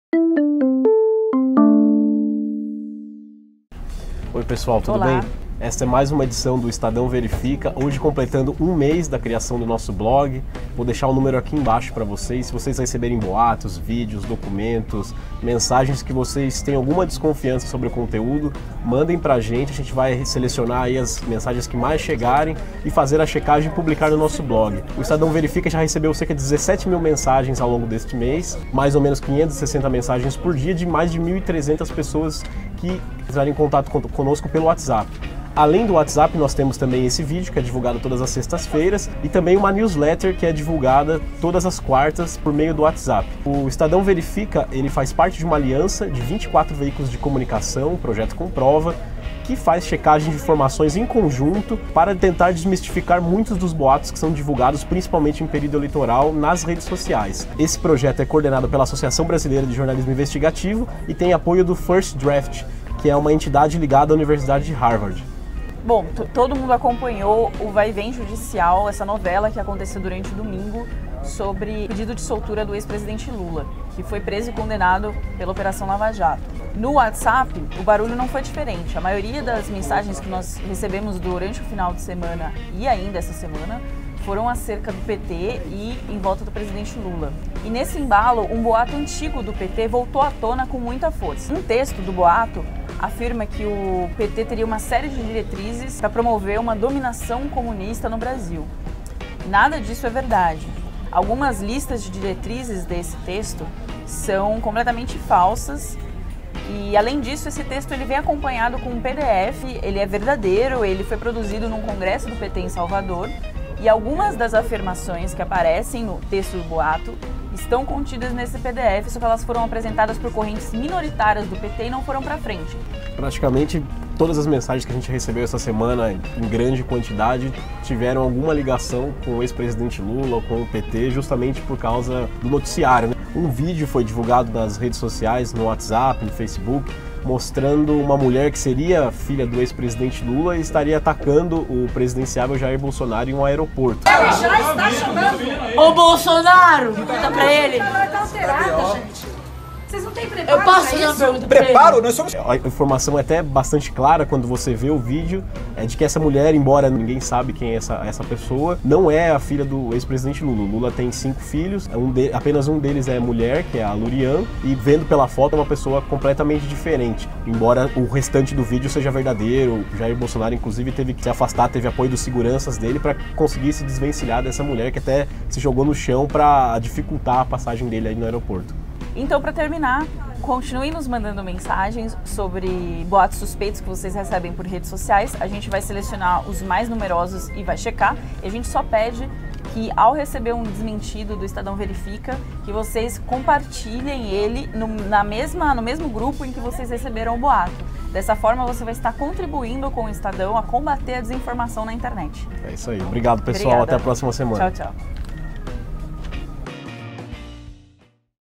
Estadão Verifica #4 - Um mês de blog, congresso do PT e filha do Lula
Links citados:
Mensagem mentirosa diz que mulher que provocou Bolsonaro é filha de Lula

Boato circula com falso ‘plano de dominação comunista’ do PT